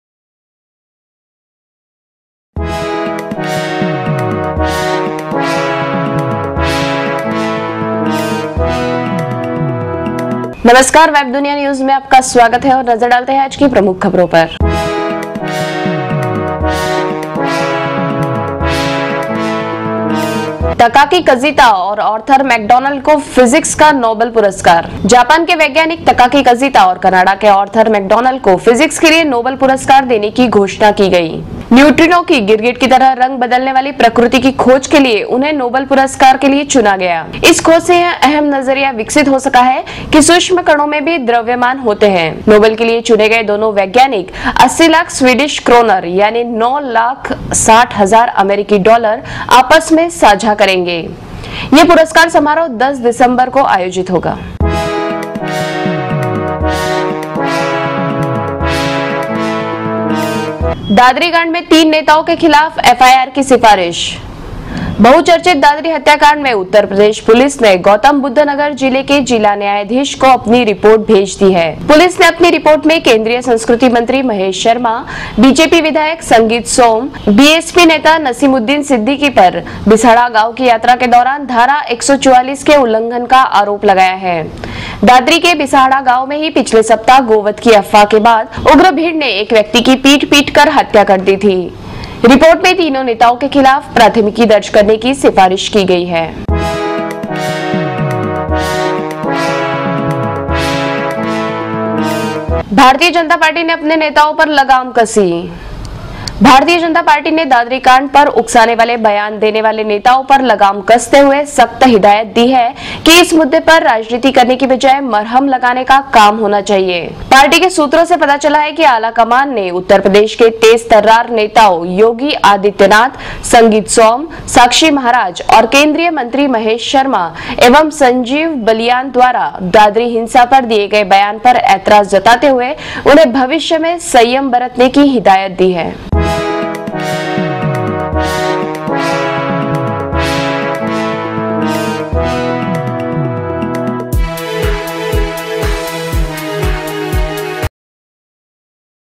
दादरी कांड में तीन नेताओं के खिलाफ FIR की सिफारिश | FIR Against Three Politicians In Dadri Kand
बहुचर्चित दादरी हत्याकांड में उत्तर प्रदेश पुलिस ने गौतम बुद्ध नगर जिले के जिला न्यायाधीश को अपनी रिपोर्ट भेज दी है। पुलिस ने अपनी रिपोर्ट में केन्द्रीय संस्कृति मंत्री महेश शर्मा, भारतीय जनता पार्टी (बीजेपी) विधायक संगीत सोम, बीएसपी नेता नसीमुद्दीन सिद्दीकी पर बिसाहड़ा गांव की यात्रा के दौरान धारा 144 के उल्लंघन का आरोप लगाया है। दादरी के बिसाहड़ा गांव में ही पिछले सप्ताह गोवध की अफवाह के बाद उग्र भीड़ ने एक व्यक्ति की पीट पीटकर हत्या कर दी थी। रिपोर्ट में तीनों नेताओं के खिलाफ प्राथमिकी दर्ज करने की सिफारिश की गई है।